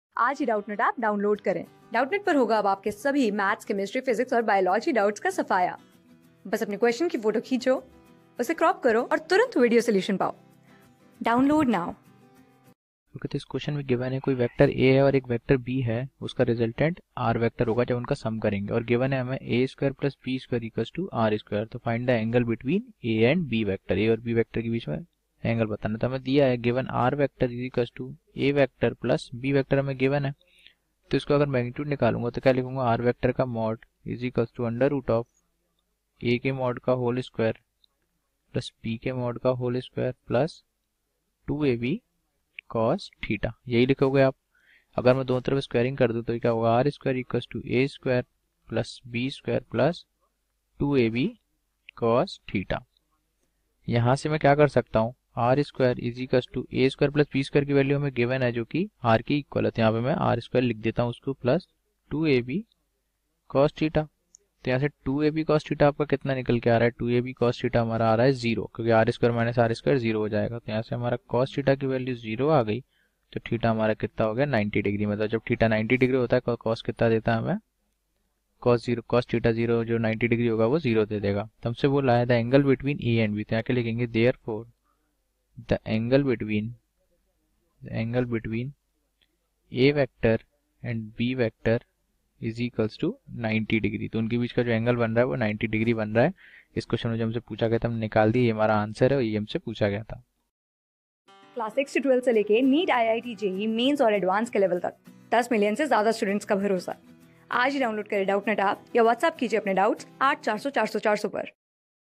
Given that vec A + vec B= vec R and A^(20 + B^(2) -R^(2), find the angle between vec A and vec...
Given that  vec A + vec B= vec R and A^(20 + B^(2) -R^(2), find the angle between  vec A and vec B .
Class: 11
Subject: PHYSICS
Chapter: KINEMATICS
Board:CBSE

👉 Have Any Query? Ask Us.